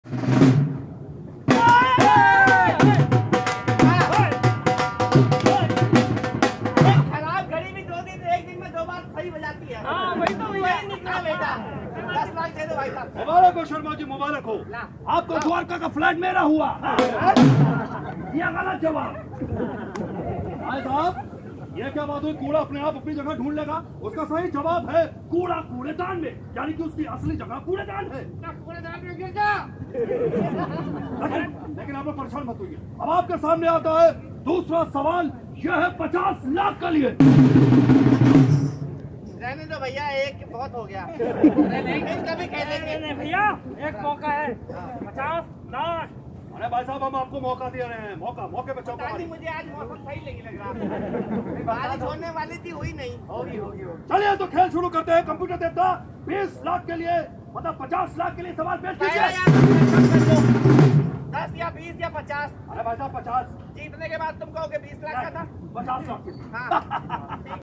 Nukkad Natak Part 7. SwachhtaPakhwada 2019
Nukkad Natak organized on 9th July'19 under SwachhtaPakhwada for mass awareness on cleanliness.